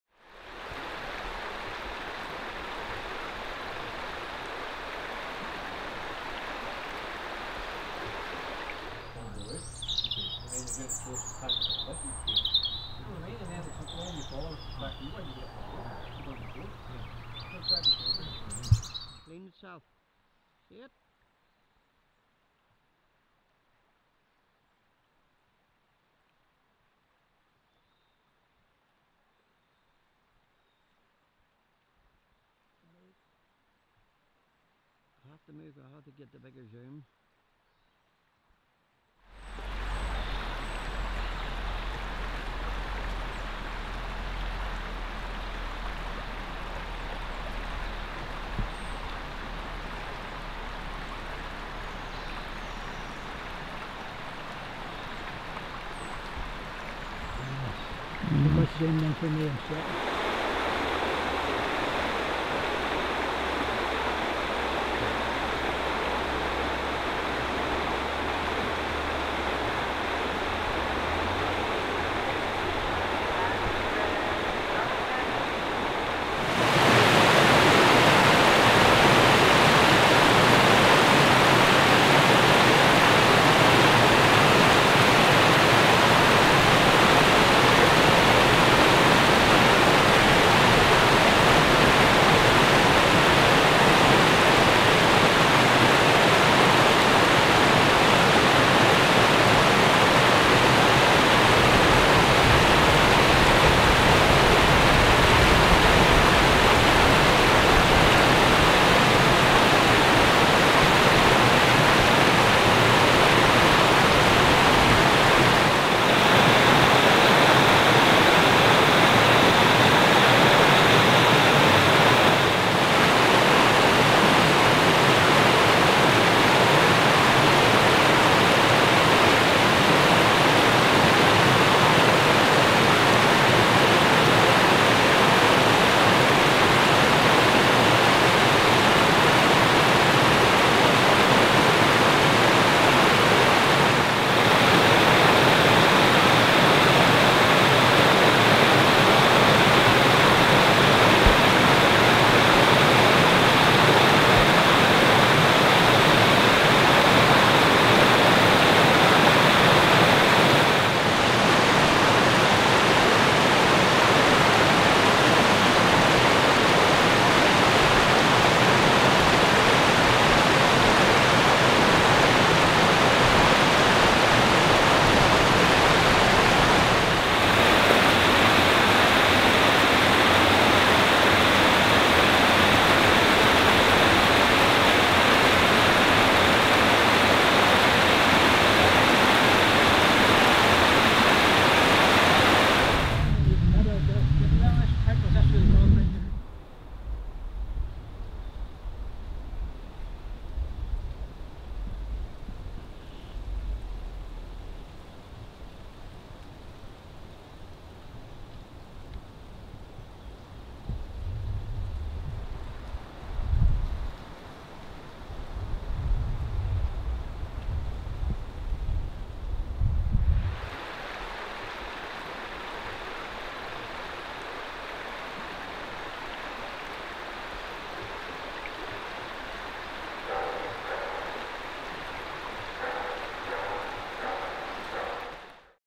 Sony FDR-AX53 Outdoors at The Clare Glen
The Clare Glen Near Tandragee Filmed with Sony FDR-AX53 and Rode Videomic Pro Rycote.If anyone would like to support me in getting a Semi Professional Camcorder Please do at